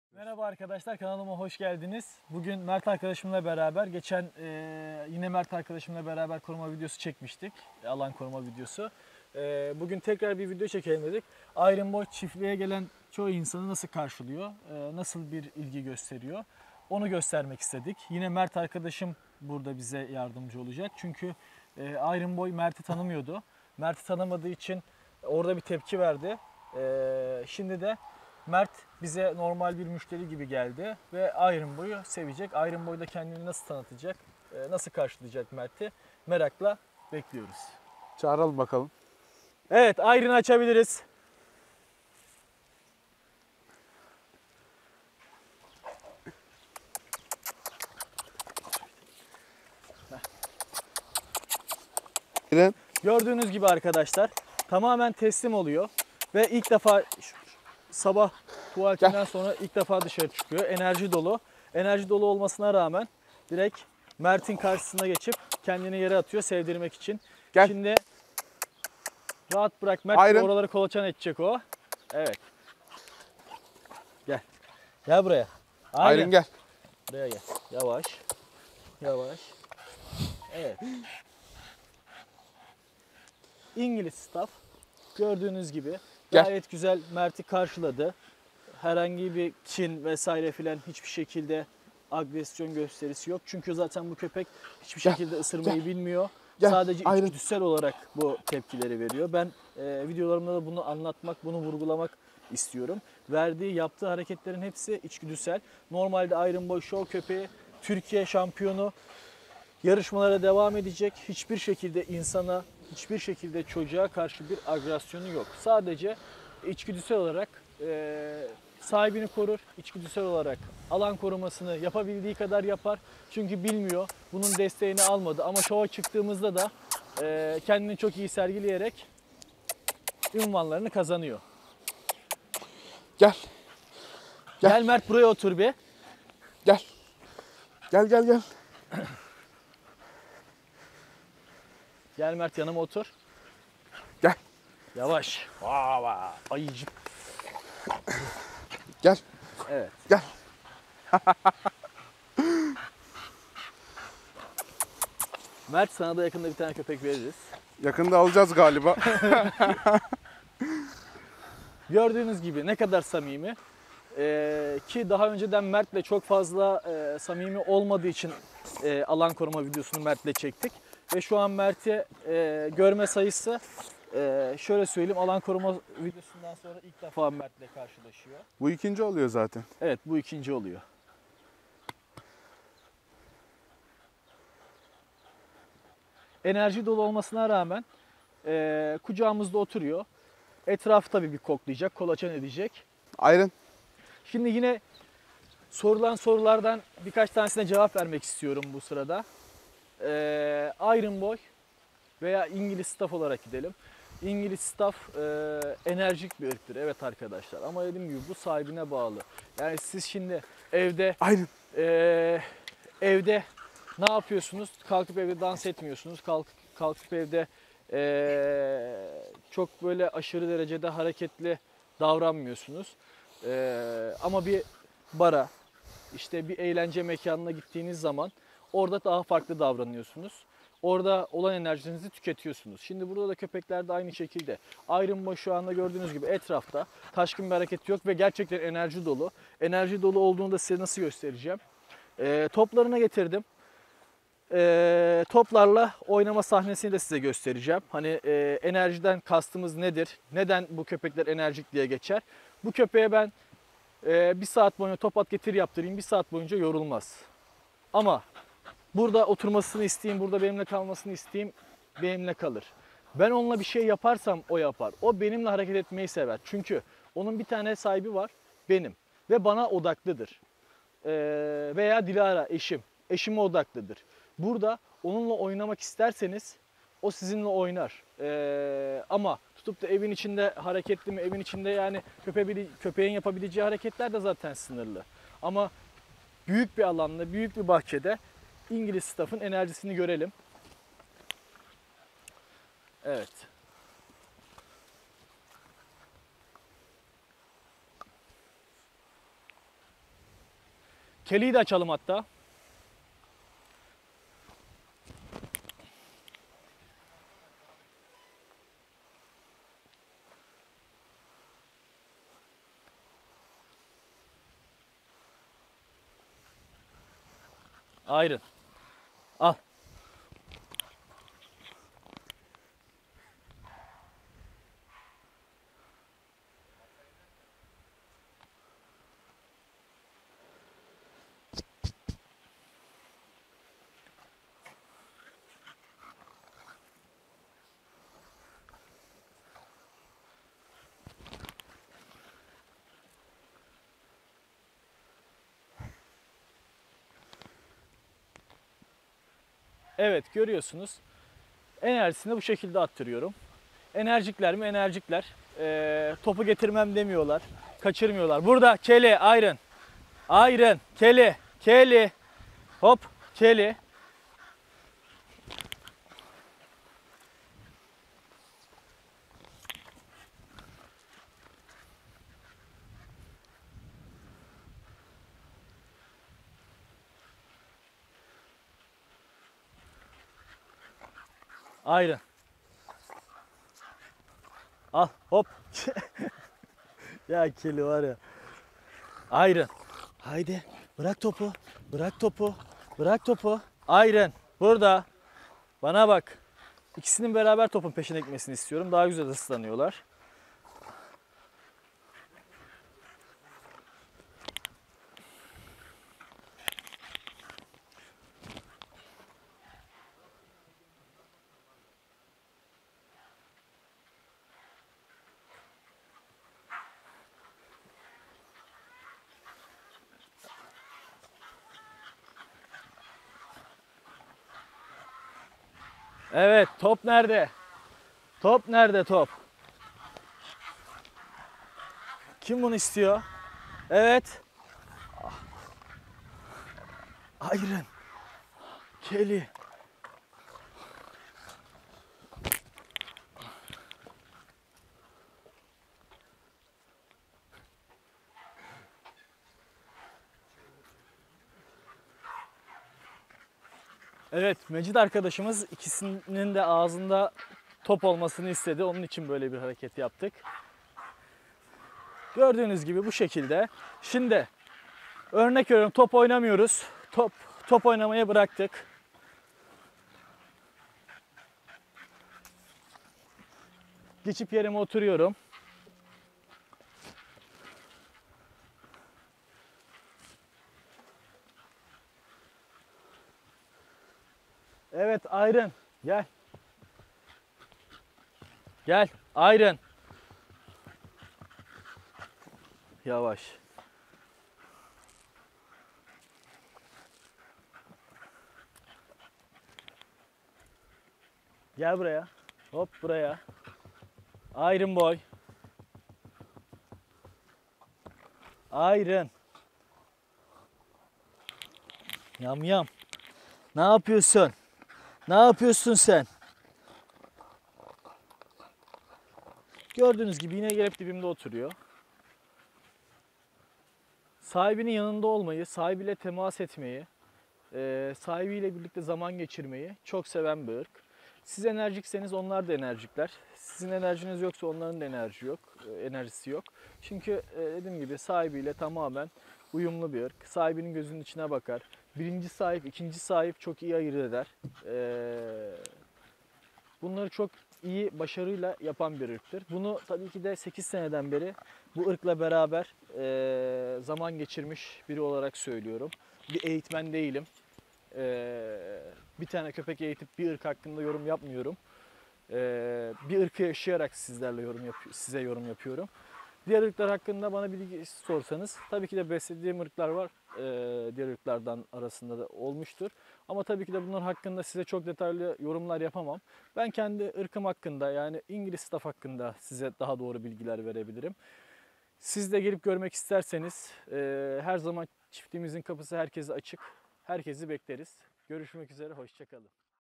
İNGİLİZ STAFF'IN İLK GÖRÜŞMEDE Kİ TEPKİLERİ
Daha iyi ve daha uzun videolarımız gelecektir bunun için

(İNGİLİZ  STAFF) STAFFORDSHİRE BULL TERRİER Irkı ev içinde çok uyumlu,zeki,eğitime çok yatkın,çocuklarla çok iyi geçinen,aile bireylerinin hepsine uyum sağlayan,sevecen,eğlenceli,mutlu bir ırktır.

Sormak istediğiniz sorular ve daha fazla bilgi için,
Facebook: Özgür Oğuz (Staffturk Kennel)